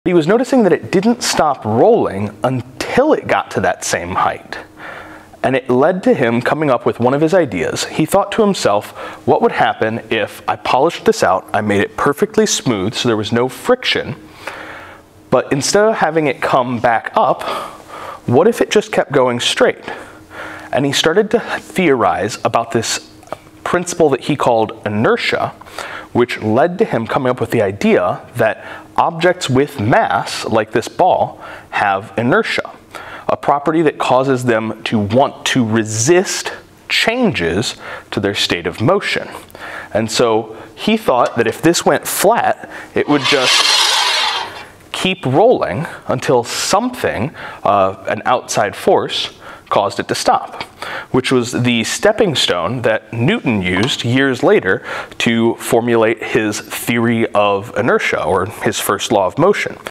What is Inertia and Who Gave Newton the Idea for His 1st Law?
Galileo discovered inertia using a track with two sides having different slopes. He found that a ball will roll until it reaches the same height on the other side.